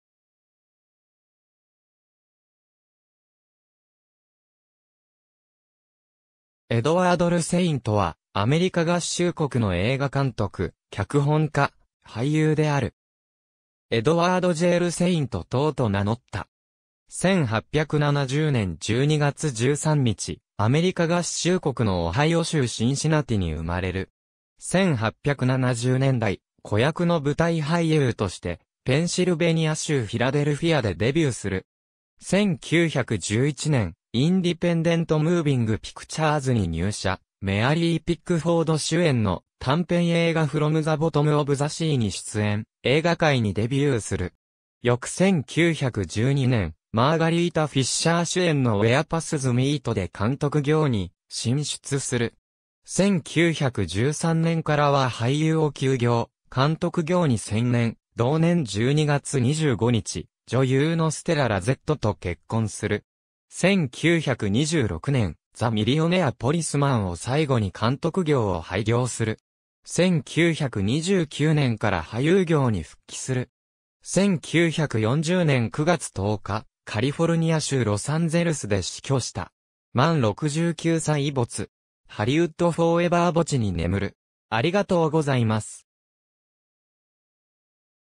エドワード・ルセイント
エドワード・ルセイント（Edward LeSaint, 1870年12月13日 - 1940年9月10日）は、アメリカ合衆国の映画監督、脚本家、俳優である。
エドワード・J・ル・セイント（Edward J. Le Saint）等と名乗った。
1870年12月13日、アメリカ合衆国のオハイオ州シンシナティに生まれる。
1870年代、子役の舞台俳優として、ペンシルベニア州フィラデルフィアでデビューする。
1911年（明治44年）、インディペンデント・ムーヴィング・ピクチャーズに入社、メアリー・ピックフォード主演の短篇映画 From the Bottom of the Sea に出演、映画界にデビューする。
翌1912年（明治45年）、マーガリータ・フィッシャー主演の Where Paths Meet で監督業に進出する。
1913年（大正2年）からは俳優を休業、監督業に専念、同年12月25日、女優のステラ・ラゼット（ステラ・ルセイント）と結婚する。
1926年（大正15年）、 The Millionaire Policeman を最後に監督業を廃業する。
1929年（昭和4年）から俳優業に復帰する。
1940年（昭和15年）9月10日、カリフォルニア州ロサンゼルスで死去した。
満69歳没。
ハリウッド・フォーエヴァー墓地に眠る。